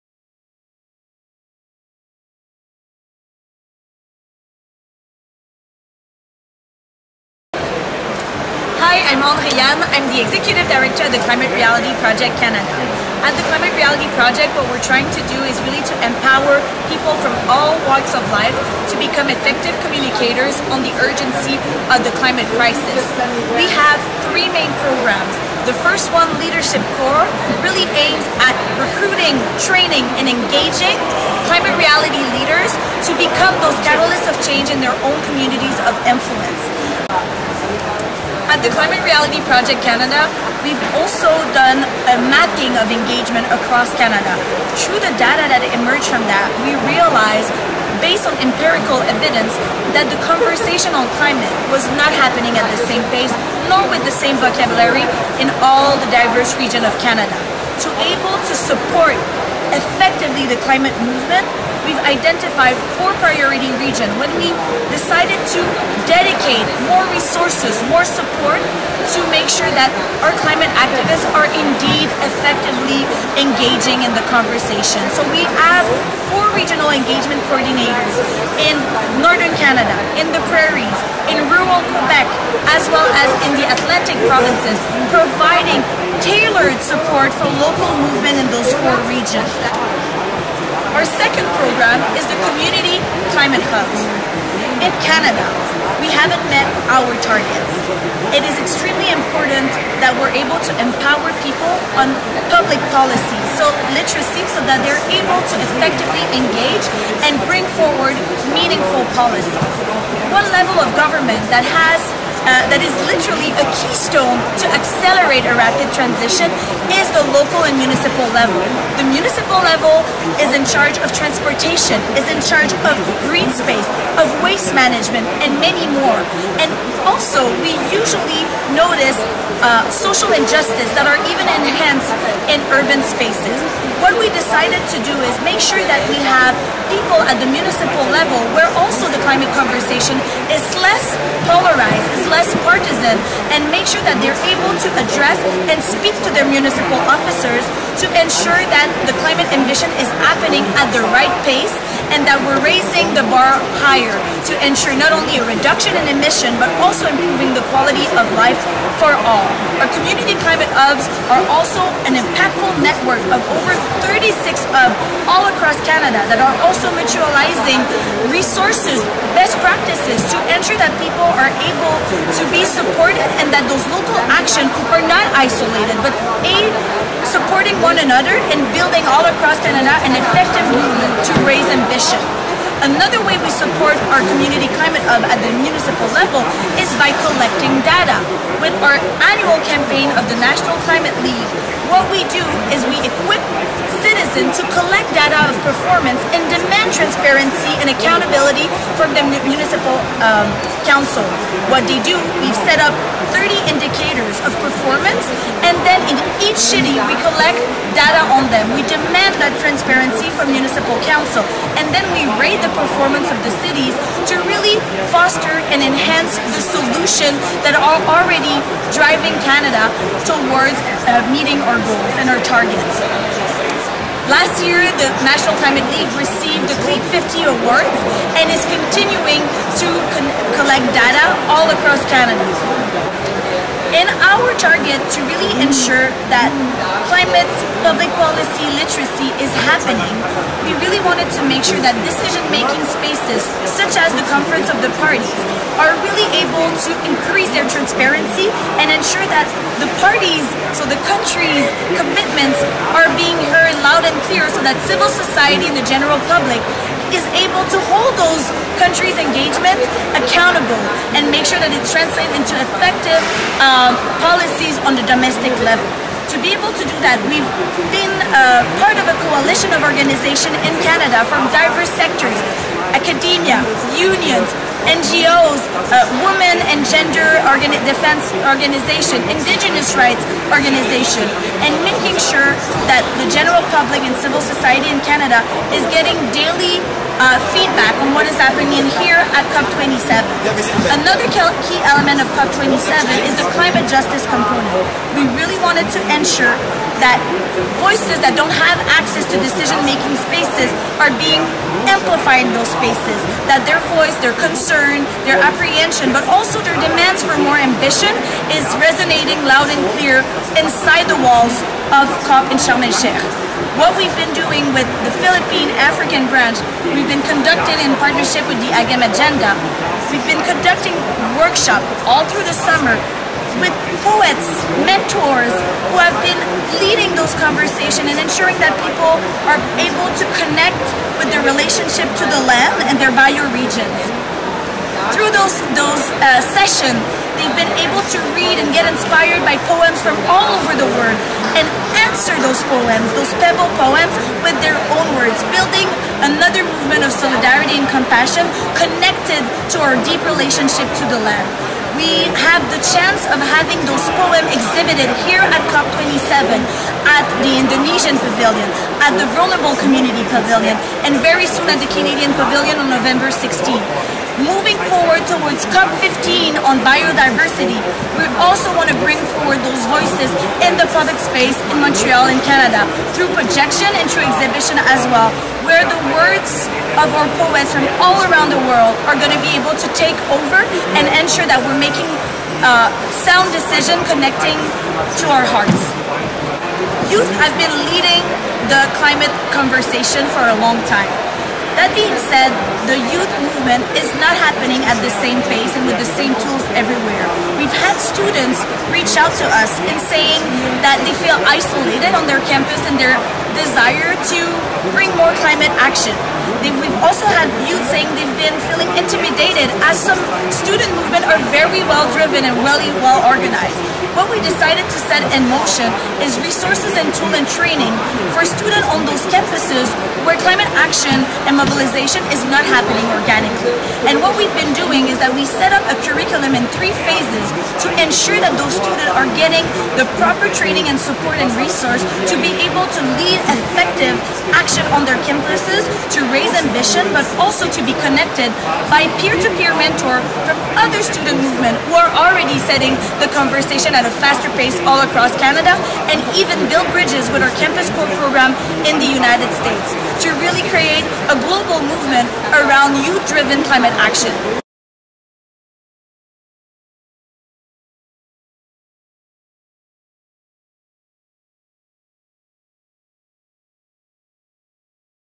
COP 27: International Climate Action in Canada
At this year’s COP, we asked Climate Reality Leaders and branches about their take on COP 27. André-Yanne Parent, executive director of Climate Reality Canada, joins us to discuss solutions, hope, and the future of international climate action.

Together, we are the solution.

Watch all the interviews now on our channel.